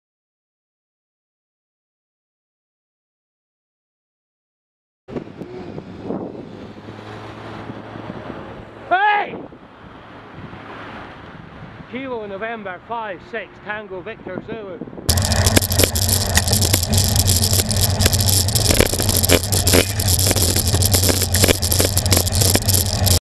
KN56TVZ close overtake on roundabout
Must have saved a tenth of a second, perhaps two, there.

Wednesday September 3rd 2014 approx 17:15